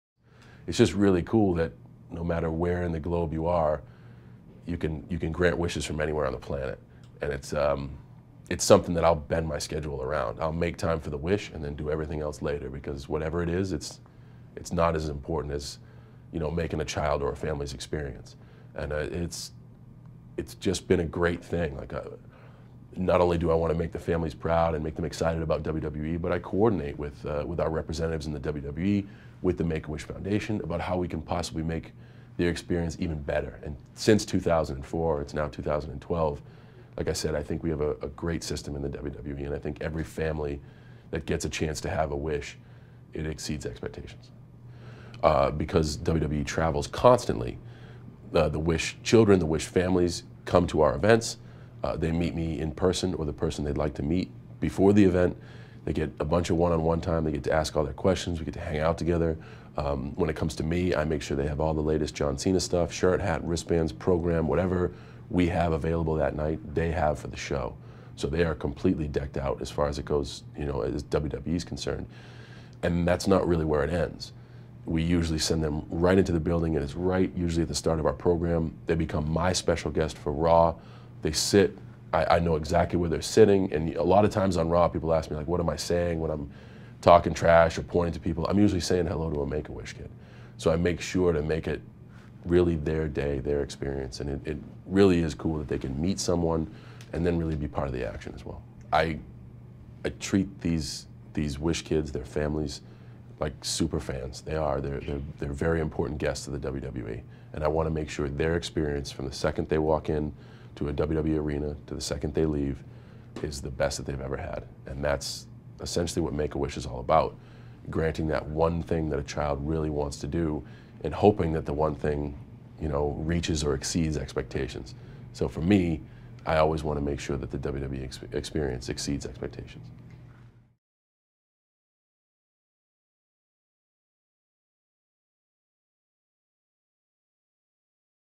John Cena on making the WWE experience special for Wish Kids
WWE Superstar John Cena has been granting wishes for Make-A-Wish since 2004. Here he talks about making the wish experience special for these WWE SuperFans and why he puts wish granting first as he travels the globe.